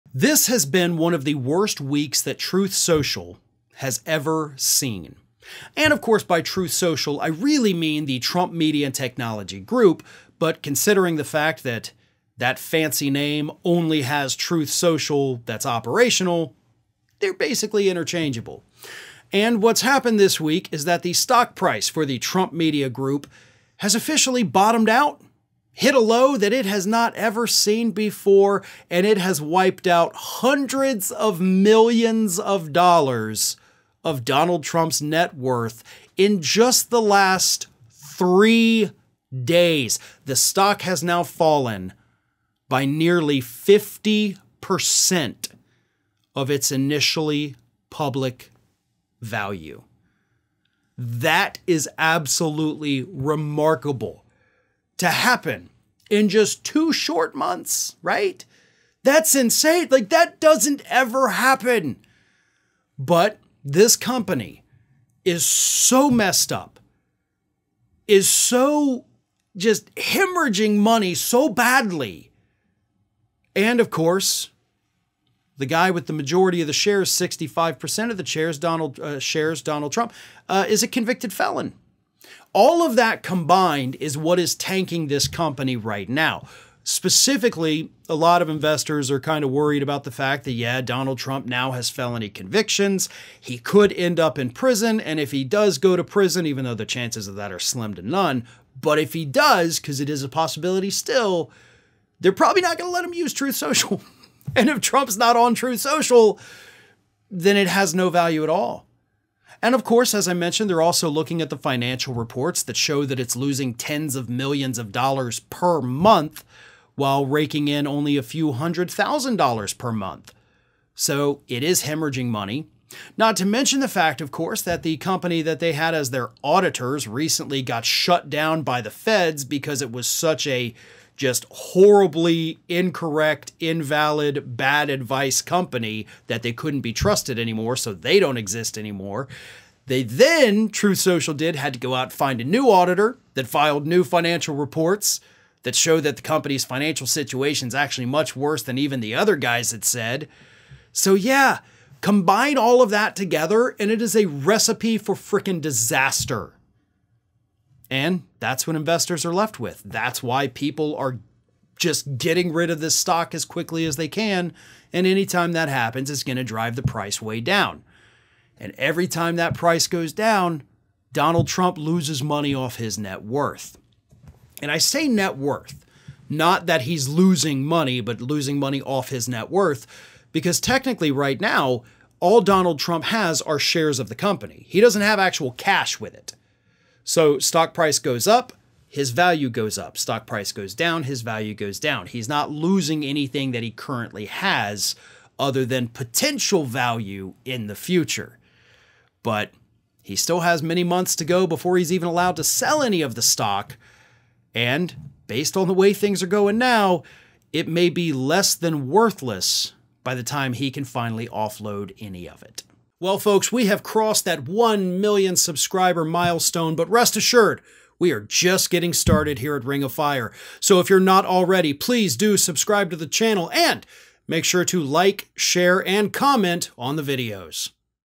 Trump Loses Hundreds Of Millions As Truth Social Stock Hits Rock Bottom
The stock price for the Trump Media & Technology Group has bottomed out this week, hitting lows that we haven't seen before. The stock has now lost nearly half of its original value, marking the largest decrease in value since it was initially offered. Because of the drop, Donald Trump himself has lost hundreds of millions of dollars from his net worth this week alone, and it doesn't look like things will be getting better any time soon. Ring of Fire's Farron Cousins explains why this is happening.

This has been one of the worst weeks that Truth Social has ever seen. And of course, by truth social, I really mean the Trump media and technology group. But considering the fact that that fancy name only has truth social, that's operational, they're basically interchangeable. And what's happened this week is that the stock price for the Trump Media group has officially bottomed out, hit a low that it has not ever seen before, and it has wiped out hundreds of millions of dollars of Donald Trump's net worth in just the last three days. The stock has now fallen by nearly 50% of its initially public value. That is absolutely remarkable to happen in just two short months, right? That's insane. Like that doesn't ever happen. But this company is so messed up, is so just hemorrhaging money so badly. And of course, the guy with the majority of the shares, 65% of the shares, Donald, uh, shares Donald Trump, uh, is a convicted felon.

All of that combined is what is tanking this company right now, specifically, a lot of investors are kind of worried about the fact that yeah, Donald Trump now has felony convictions. He could end up in prison and if he does go to prison, even though the chances of that are slim to none, but if he does, 'cause it is a possibility still they're probably not gonna let him use truth social. And if Trump's not on Truth Social, then it has no value at all. And of course, as I mentioned, they're also looking at the financial reports that show that it's losing tens of millions of dollars per month while raking in only a few hundred thousand dollars per month. So it is hemorrhaging money. Not to mention the fact, of course, that the company that they had as their auditors recently got shut down by the feds because it was such a just horribly incorrect, invalid, bad advice company that they couldn't be trusted anymore.

So they don't exist anymore. They then True Social did had to go out and find a new auditor that filed new financial reports, that showed that the company's financial situations actually much worse than even the other guys had said. So yeah, combine all of that together and it is a recipe for fricking disaster. And that's what investors are left with. That's why people are just getting rid of this stock as quickly as they can. And anytime that happens, it's gonna drive the price way down. And every time that price goes down, Donald Trump loses money off his net worth. And I say net worth, not that he's losing money, but losing money

Off his net worth. Because technically right now, all Donald Trump has are shares of the company. He doesn't have actual cash with it. So stock price goes up, his value goes up, stock price goes down, his value goes down. He's not losing anything that he currently has other than potential value in the future. But he still has many months to go before he is even allowed to sell any of the stock. And based on the way things are going now, it may be less than worthless by the time he can finally offload any of it.